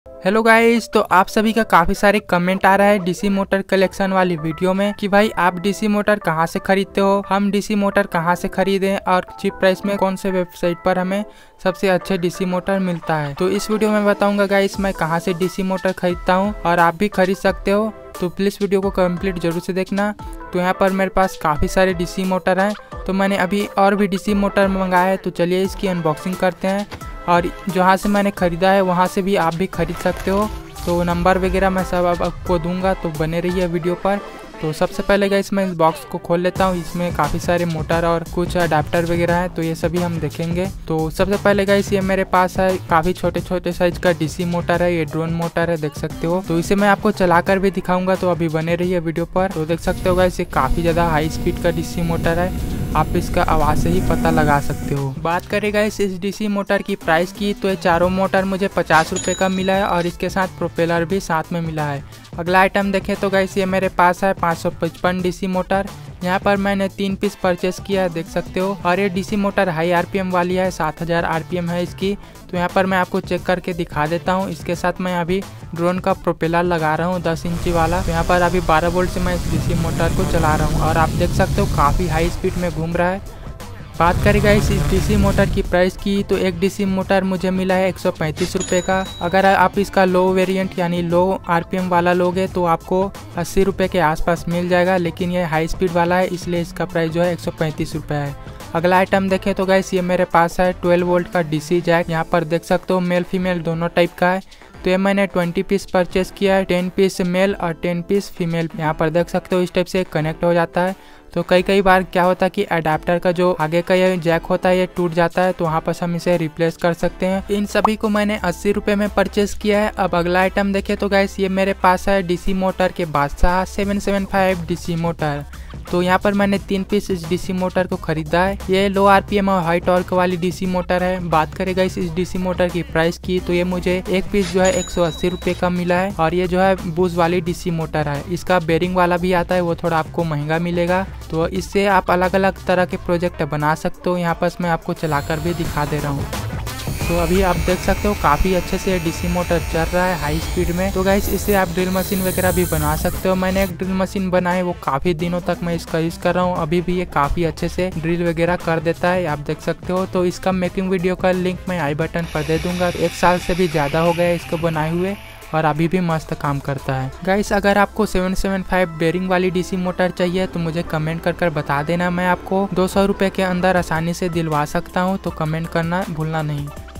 मिल गया सस्ते में RS 775 DC MOTOR Online 😱 555 DC Motor, Drone Motor at cheap price | Hacker Jp
Hello guys welcome to hacker jp. Guys is video is video mein main 775 DC motor 555 DC motor and Drone motor ki unboxing Kiya hai. DC motor se aap DIY project, DC motor life hacks, drill machine with 775 motor aur school projects aur bhi bahut kuchh banaa sakte ho. Doston main jyadatar electronic components ki shopping online hi karta hun. In DC motor ko maine bahut hi cheap price mein Buy Kiya hai.